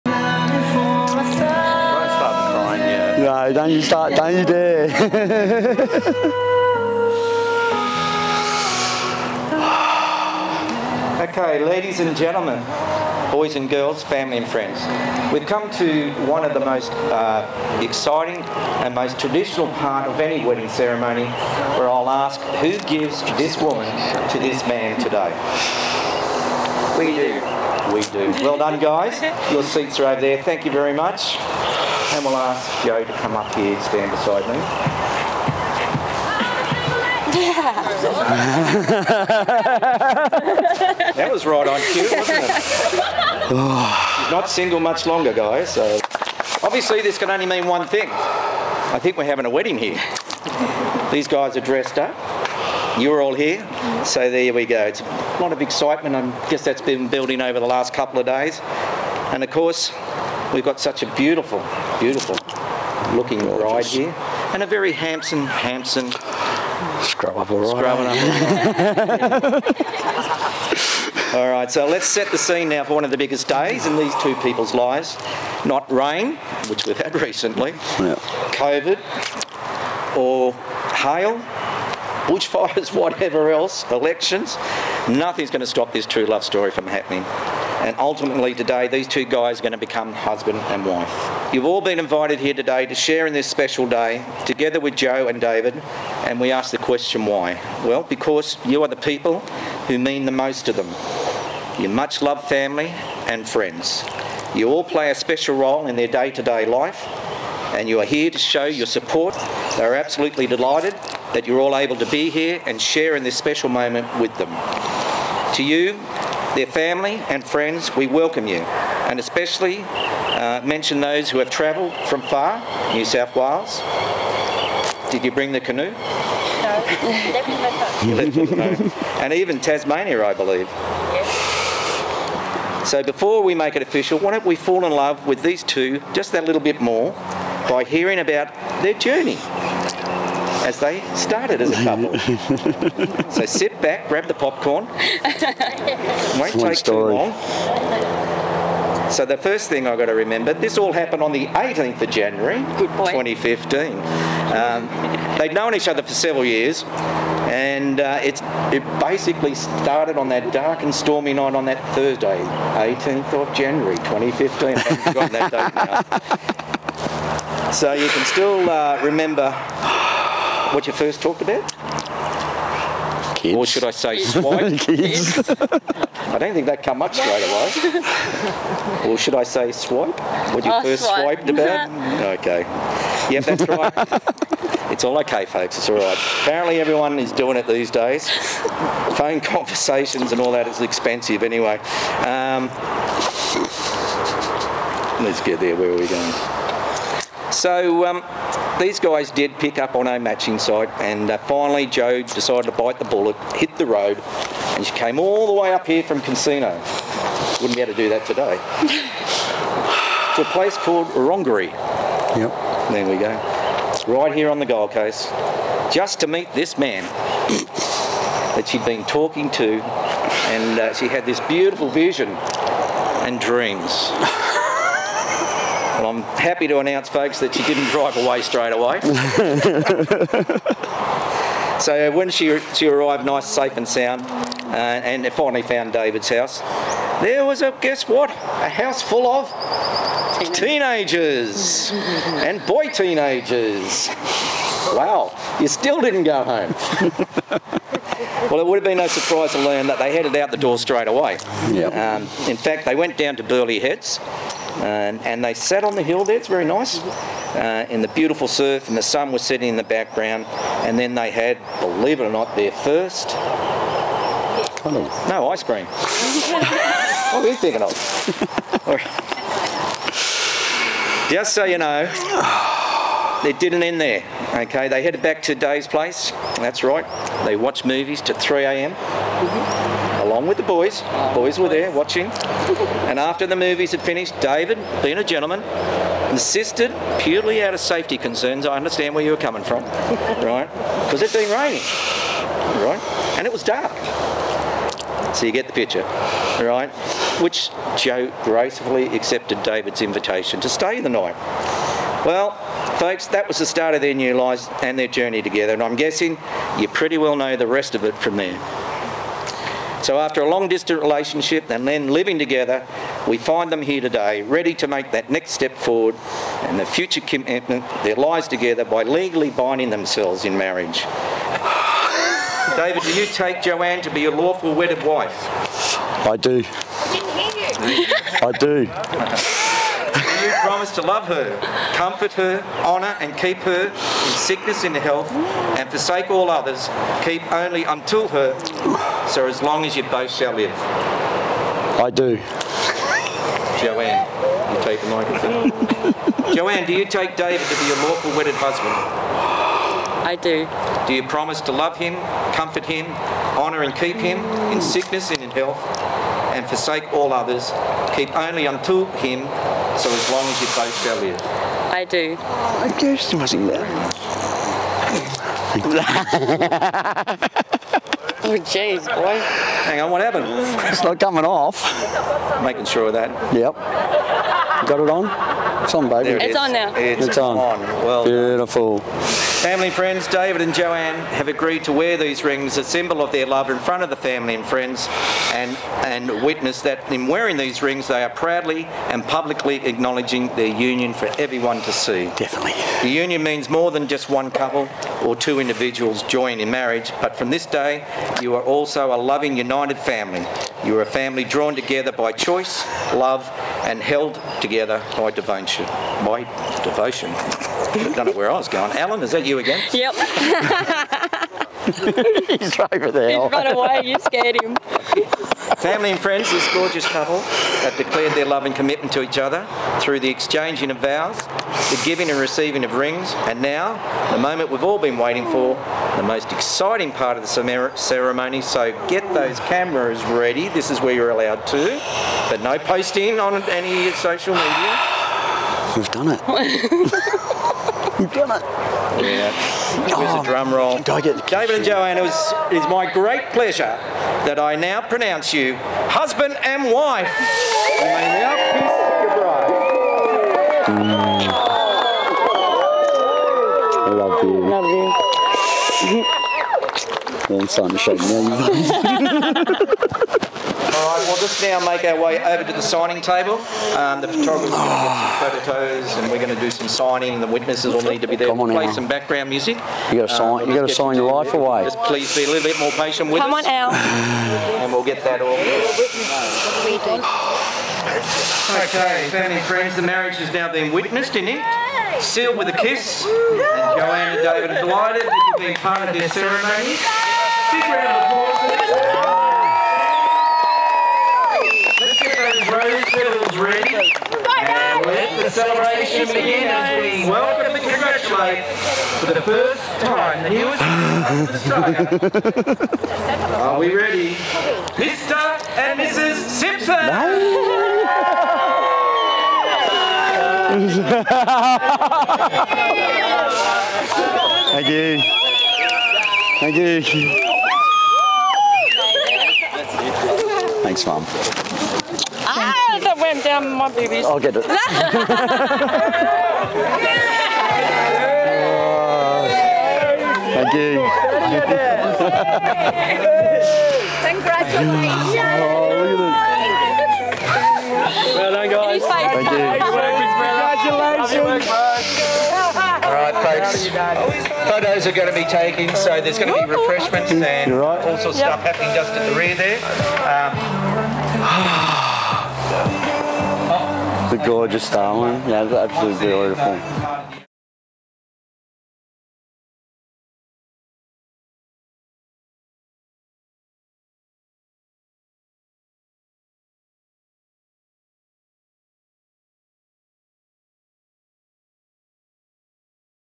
Mark Byrnes Celebrant
Joanne n David's Wedding @Coolibah Downs Private Estate, Nerang. Officiating Mark Byrnes Celebrant.